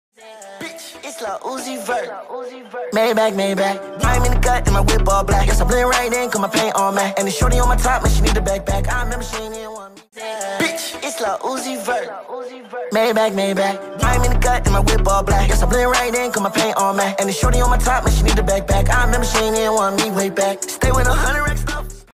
Pick My Next Intro (Vote Below)
Yea you've got 2 options. Comment 1 for option 1 and Comment 2 for option 2